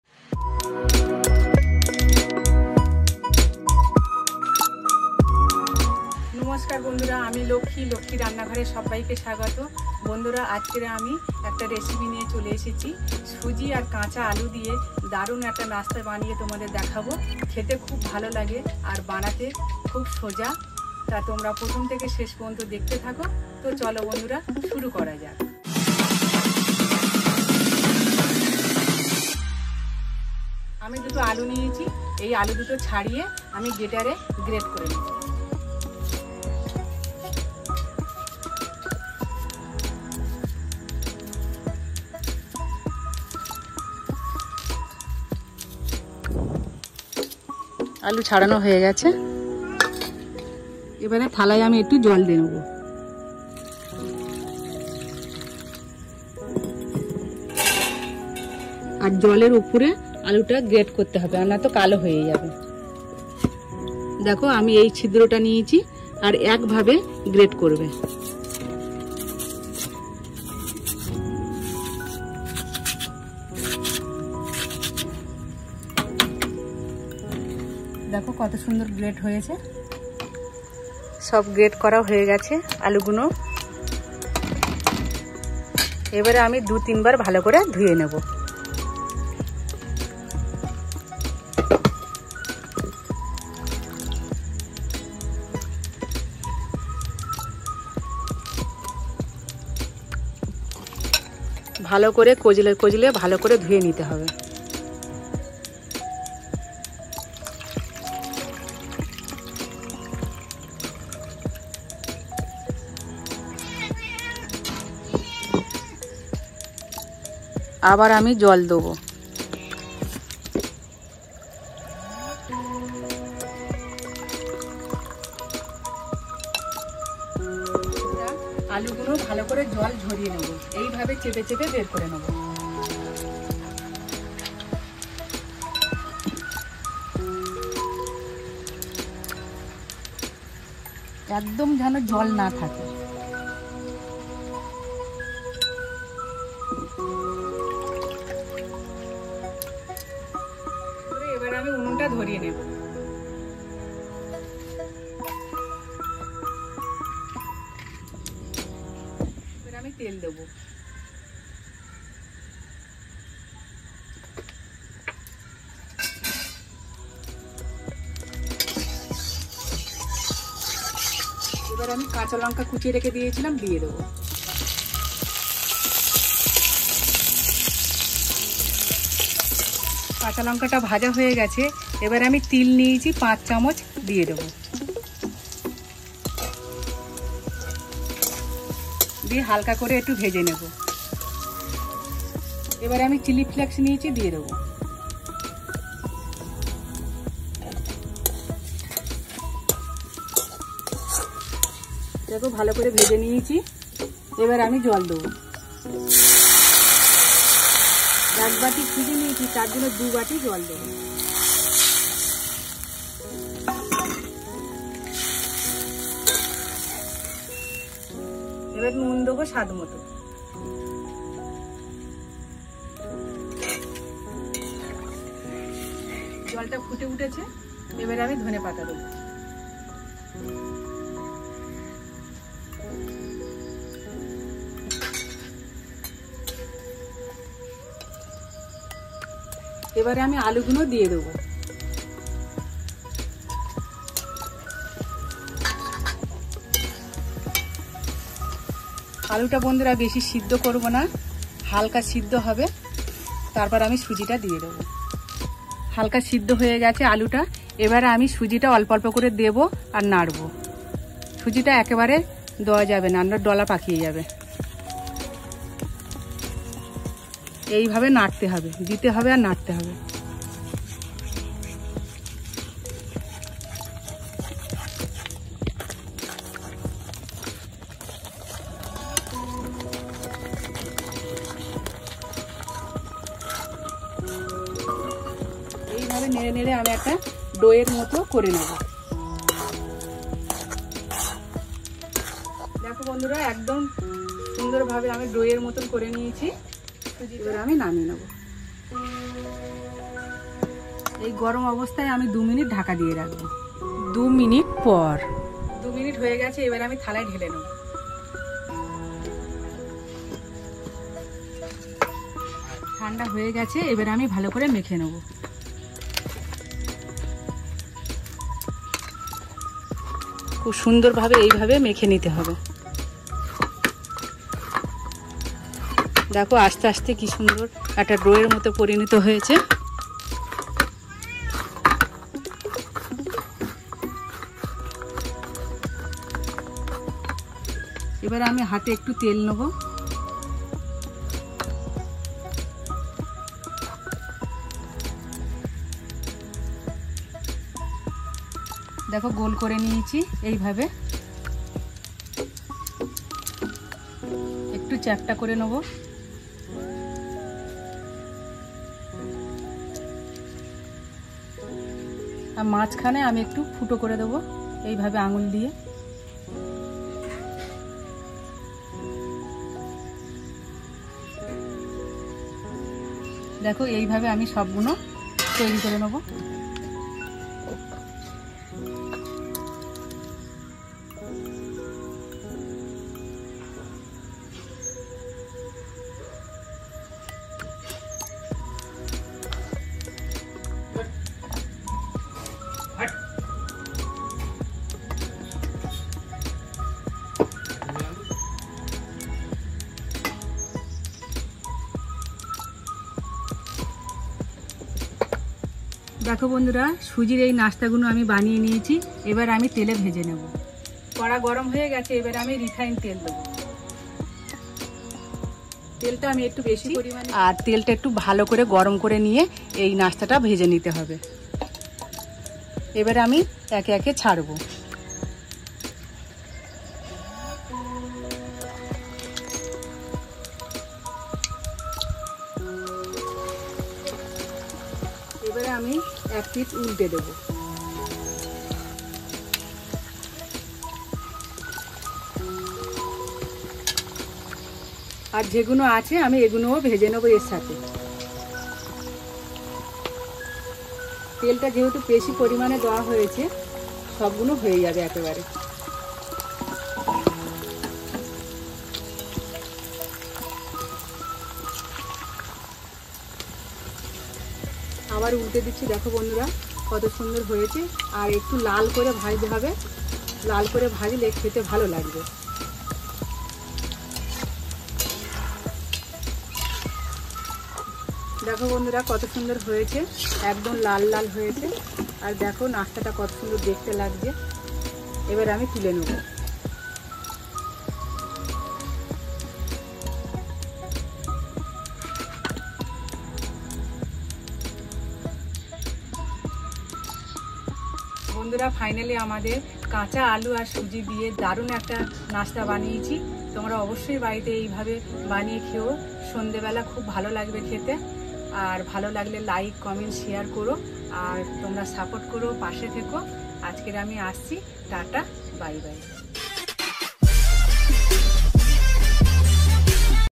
😮সুজি ও আলুর দারুণ মুচমুচে স্নাক্স একবার খেলে বারবার খেতে ইচ্ছা হবে ।
Super Crispy Suji Aloo Snacks Recipe in Bengali . Suji Aloo Nasta Recipe Bangla .